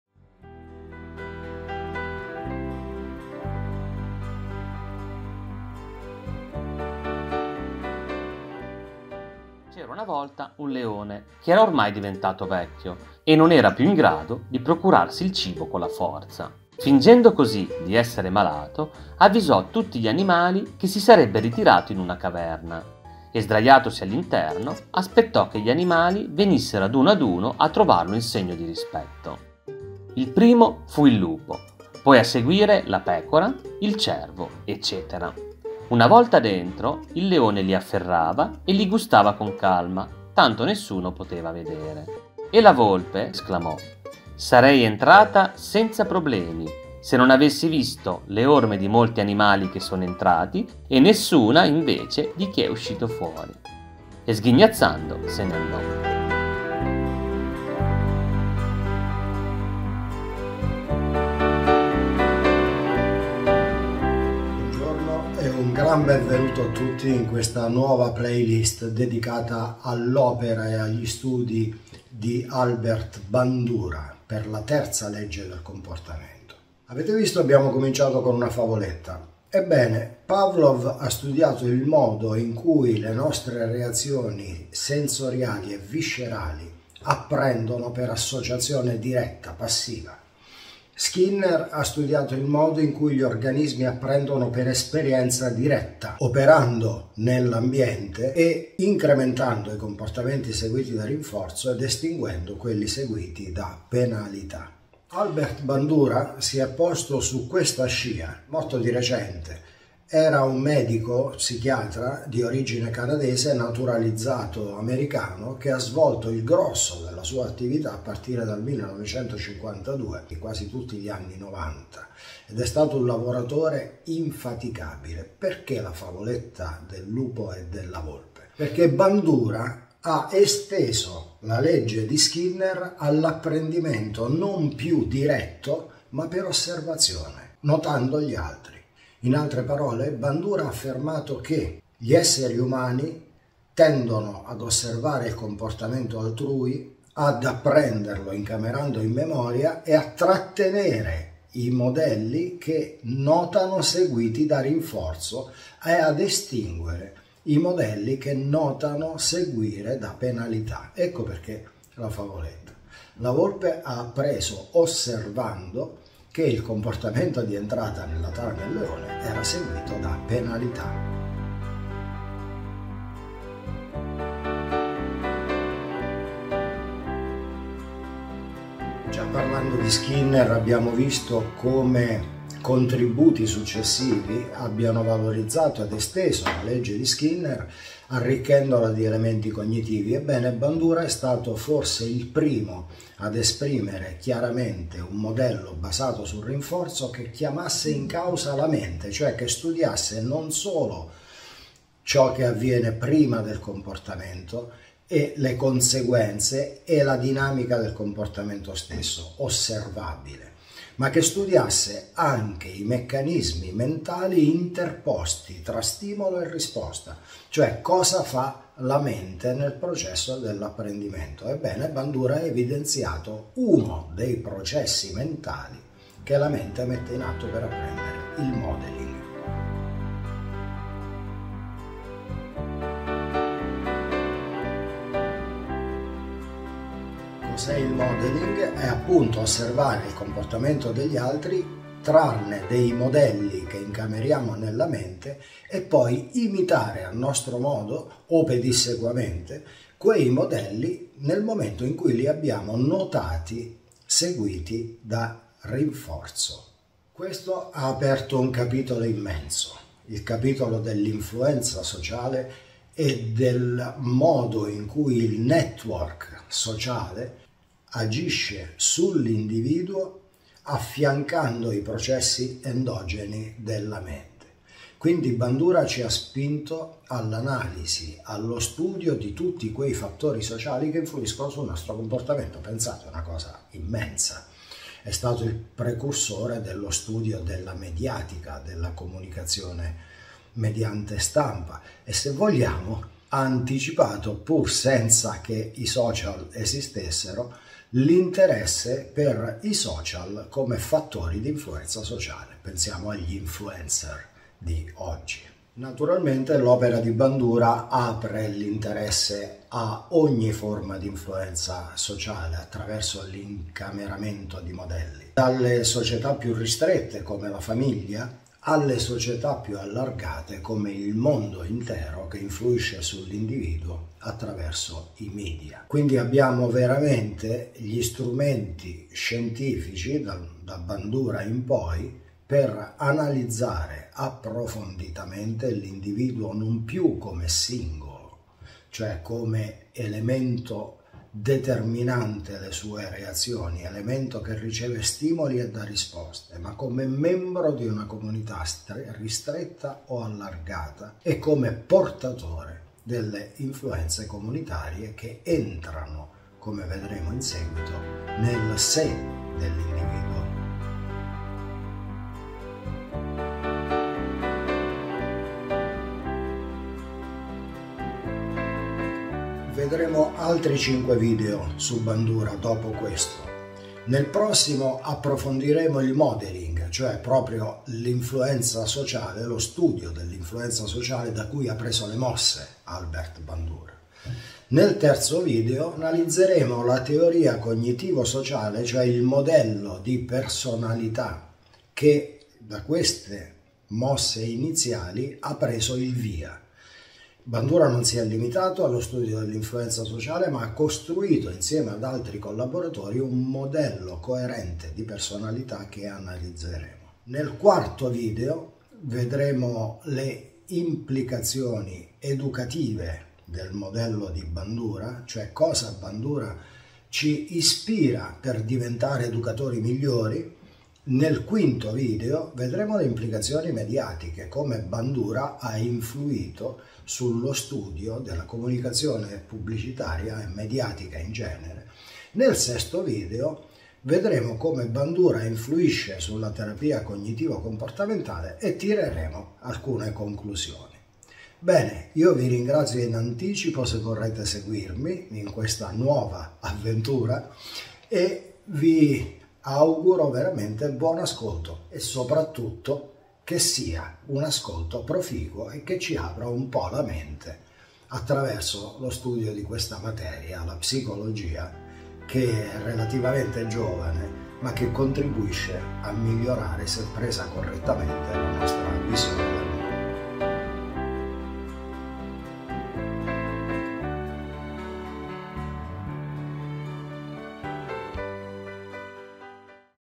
1 di 6. Legge dell'Apprendimento Sociale di Bandura. Trailer
Un'introduzione a questa nuova playlist che tratterà in modo in cui l'ambiente sociale fornisce momento per momento dei modelli che possono essere appresi per osservazione e modificare la personalità dell'essere umano.